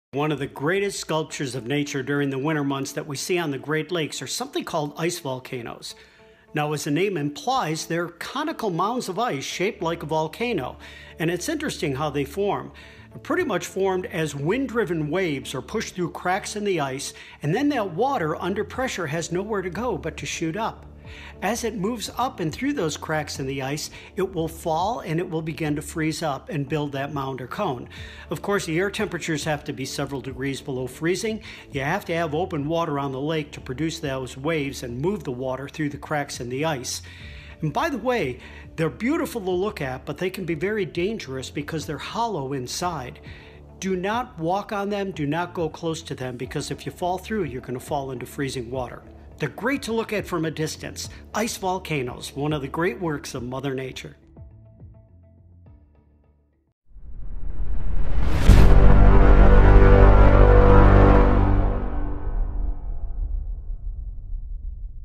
Beware of Cool Yet Dangerous Great Lakes Phenomenon
Ice Volcanoes, the Awesome Winter Wonder, Explained

An incredible sight appears on the Great Lakes during winter -- tiny volcanoes that erupt on the water. But what exactly are these ice volcanoes and how do they form? Winter weather expert, Tom Niziol, explains.